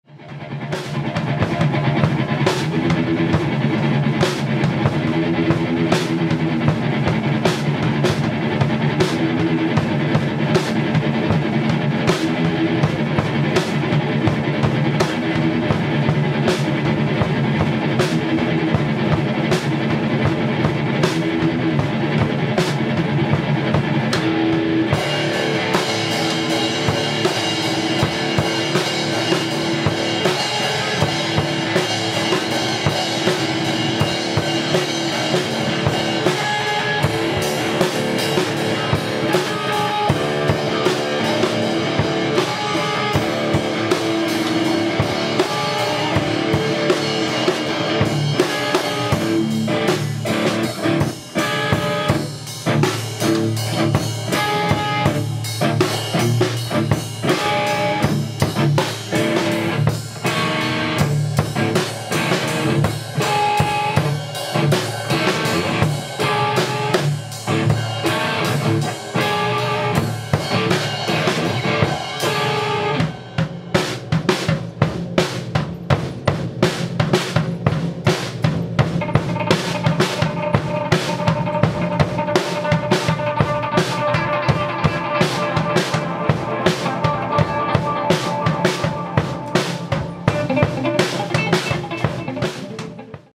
Boeuf Adèle et Tom
En souvenir d'un anniversaire bien trop cool à mon goût!
Petit boeuf improvisé par Tom  et Adèle